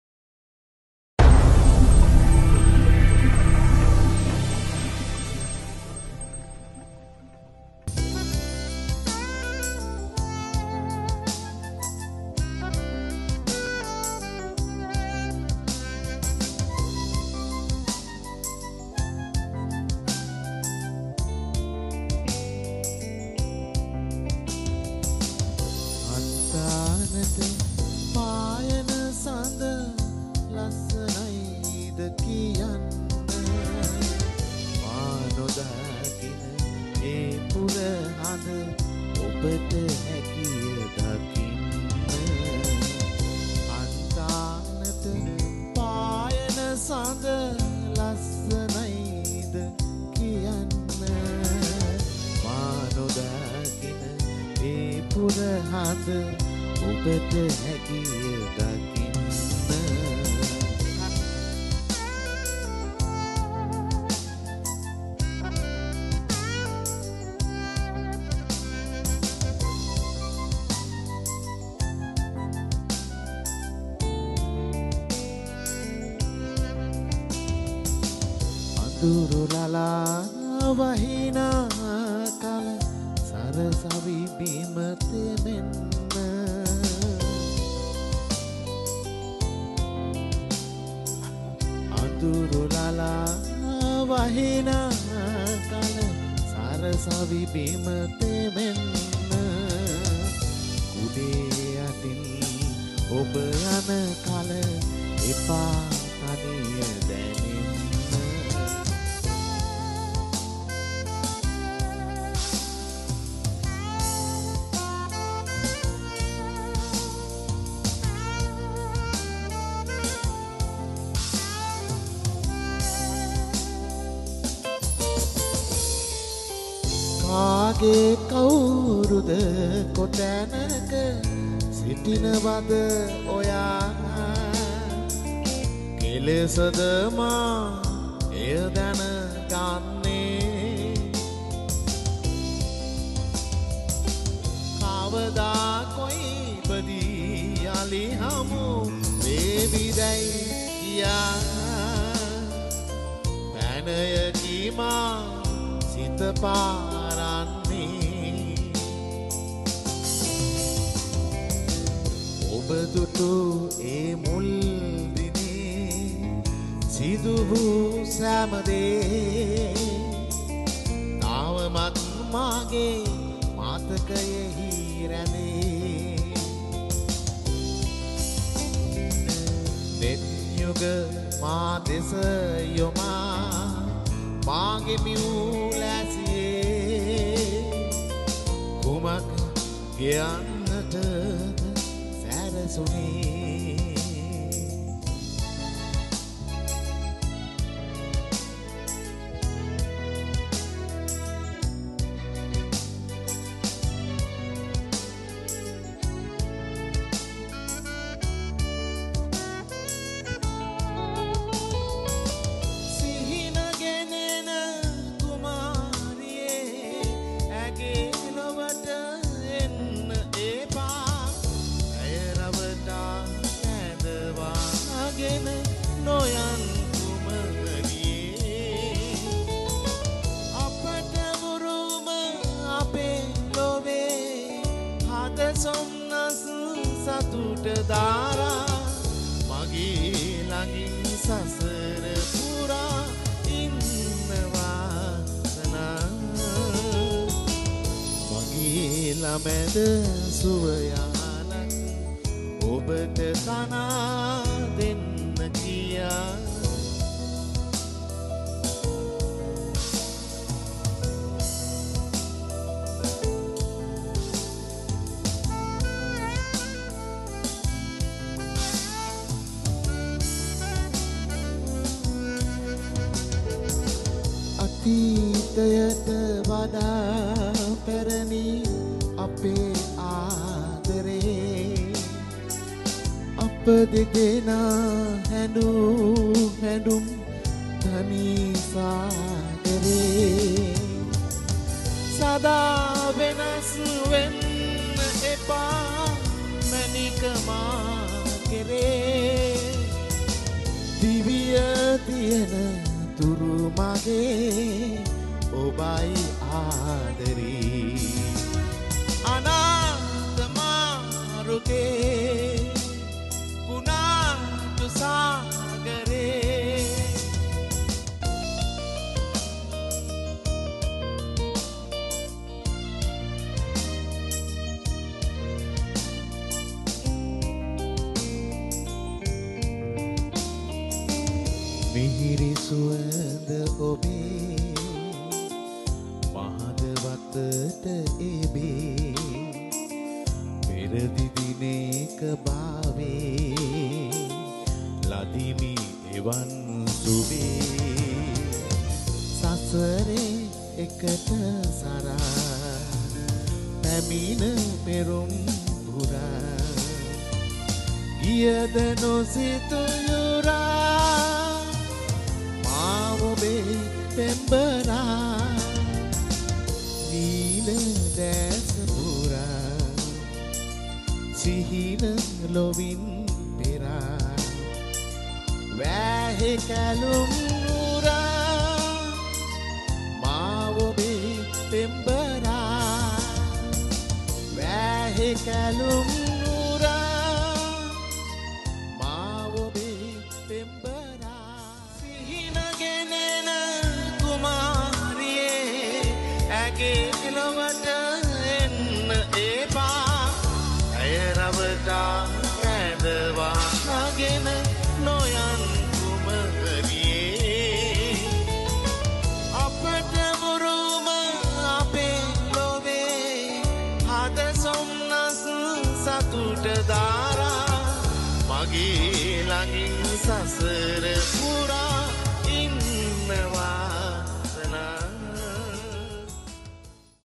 Wedding mashup LEORA Entertainments new combo pack 0779254689
We leora entertainments  introdusing new combo specially for wedding also other kind events 2pce band with dj not just that we also do lighting and led panel
dear customer ,
                                      @ Leora
                                       @ leo2 dj
                   powered by @ mr saxo (anil wasantha)